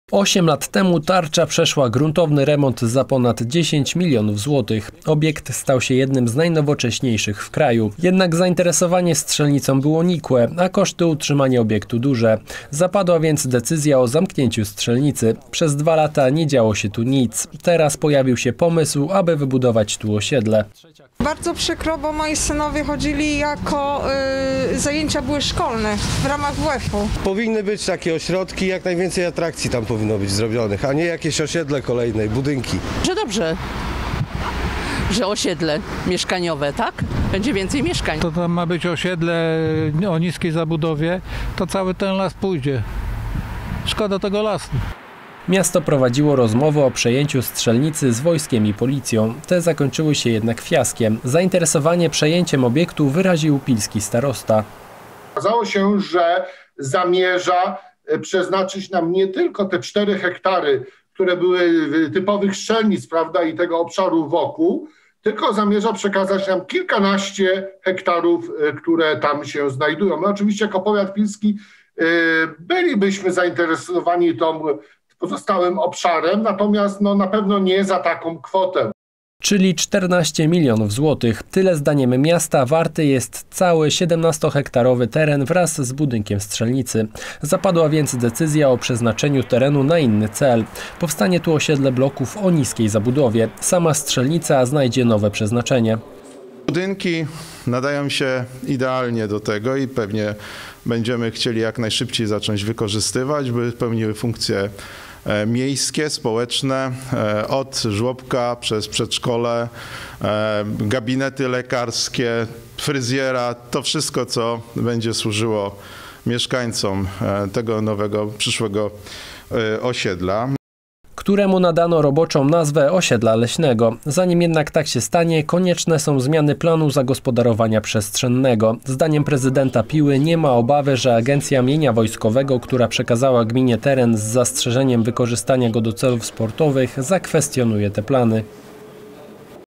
TV ASTA: Zamiast strzelnicy powstanie osiedle Leśne
Zamiast strzelnicy powstanie osiedle Leśne